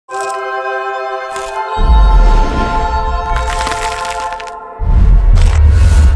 Nuevas Saladix Edición Limitada Sabor Queso
La Nueva Edición Limitada Sabor Queso es de esas ediciones que preferirías que no sean limitadas. Probalas antes de que se acaben y descubrí su irresistible sabor queso.